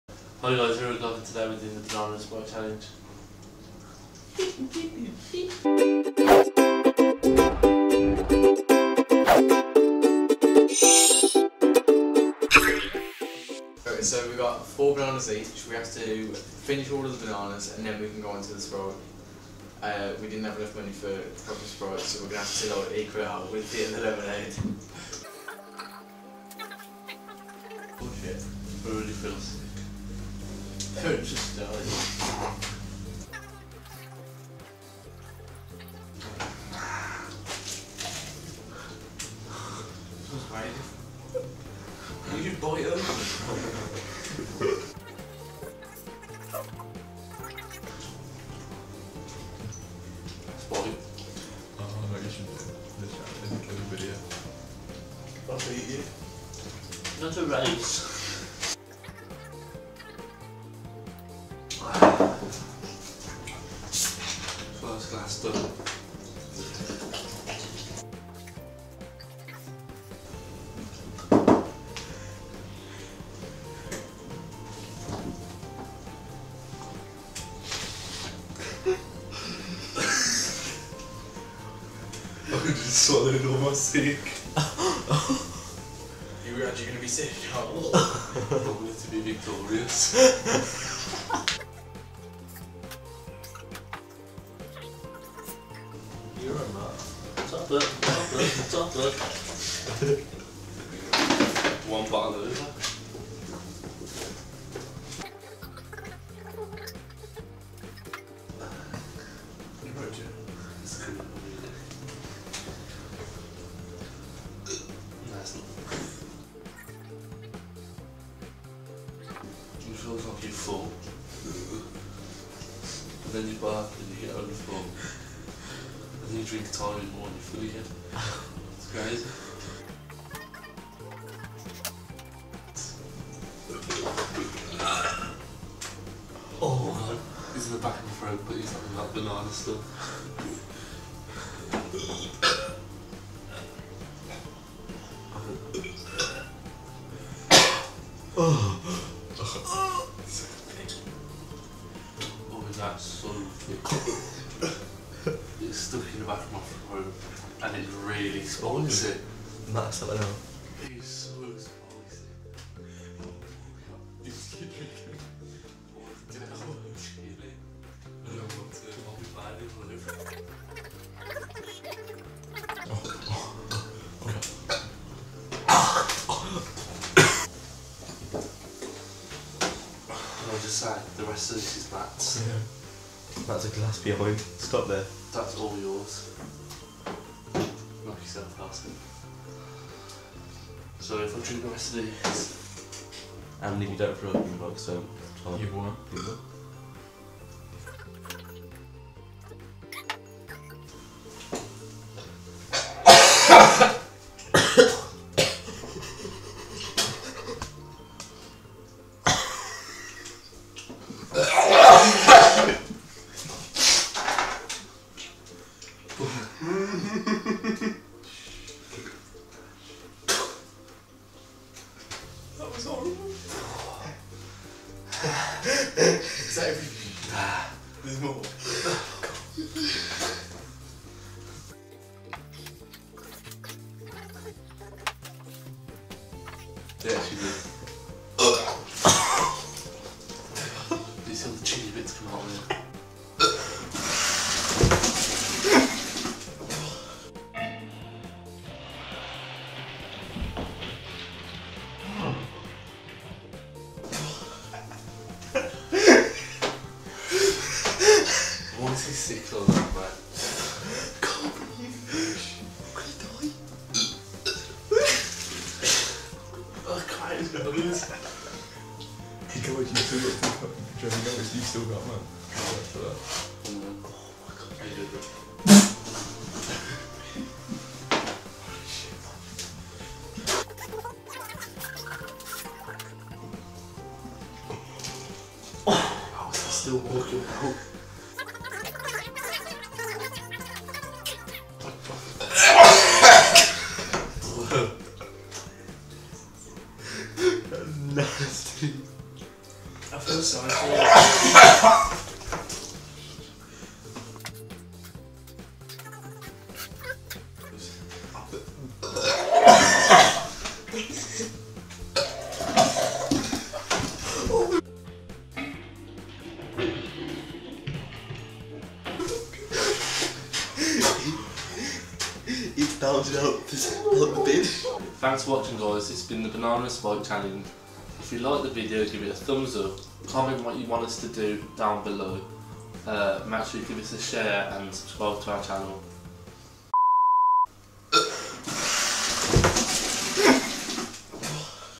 Banana & Sprite challenge *VOMIT ALERT*
Joe and Matt take on the famous Banana and sprite challenge, however they are doubling the amount. So in this challenge they attempt to finish 4 Bananas and 2 litres of sprite each...

Can they do it?